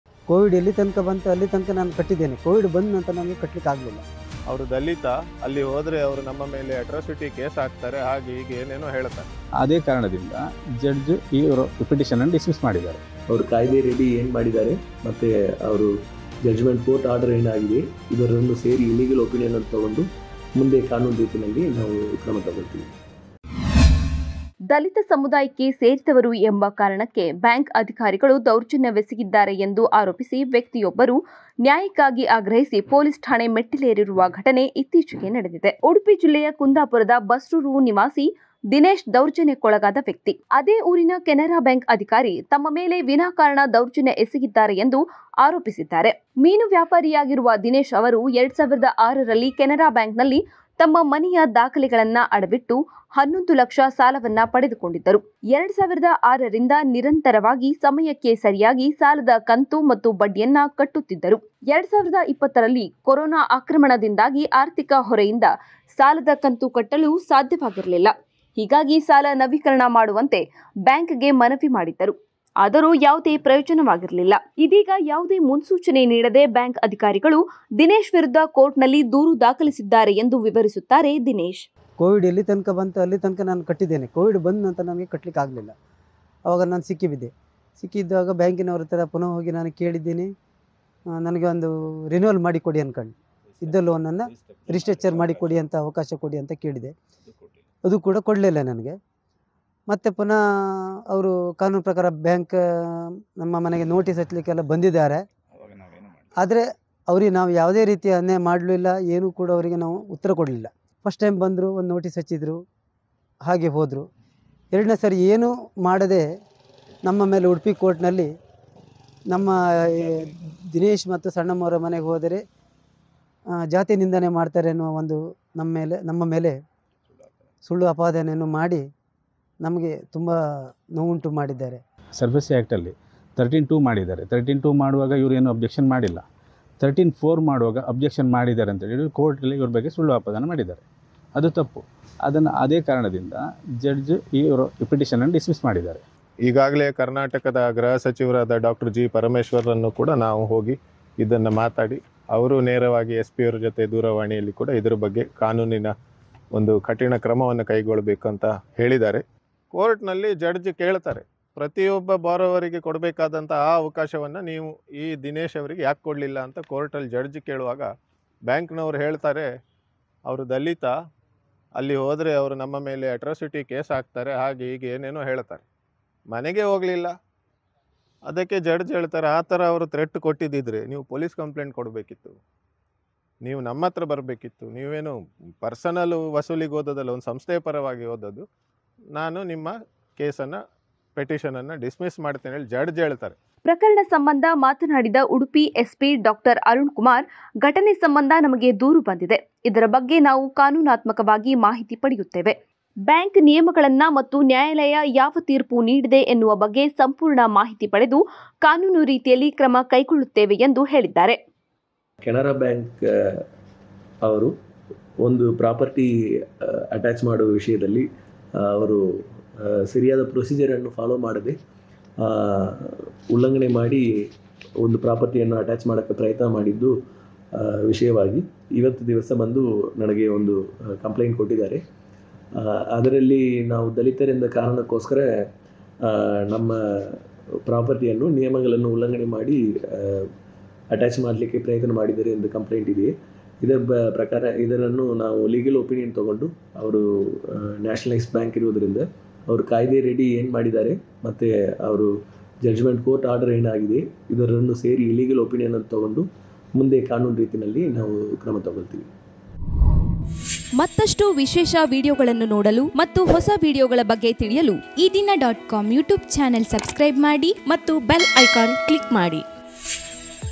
ದಲಿತ ಎಂಬ ಕಾರಣಕ್ಕೆ ಬ್ಯಾಂಕ್‌ ಅಧಿಕಾರಿಯಿಂದ ದೌರ್ಜನ್ಯ ಆರೋಪ | Dalit Live Matter | Bank I Udupi
ದಲಿತ ಸಮುದಾಯಕ್ಕೆ ಸೇರಿದವರು ಎಂಬ ಕಾರಣಕ್ಕೆ ವ್ಯಕ್ತಿಯೊಬ್ಬರ ಮೇಲೆ ಬಾಂಕ್‌ ಅಧಿಕಾರಿಗಳು ದೌರ್ಜನ್ಯ ವೆಸದಿದ್ದಾರೆ ಎಂದು ಆರೋಪಿಸಲಾಗಿದೆ. ಉಡುಪಿ ಜಿಲ್ಲೆಯ ಕುಂದಾಪುರದ ಬಸ್ರೂರು ನಿವಾಸಿ ದಿನೇಶ್‌ ಎಂಬುವವರು ಮೇಲೆ ಅಲ್ಲಿನ ಕೆನರಾ ಬ್ಯಾಂಕ್‌ ಅಧಿಕಾರಿ ದೌರ್ಜನ್ಯ ಎಸಗಿದ್ದಾರೆ ಎಂದು ಆರೋಪಿಸಲಾಗಿದೆ.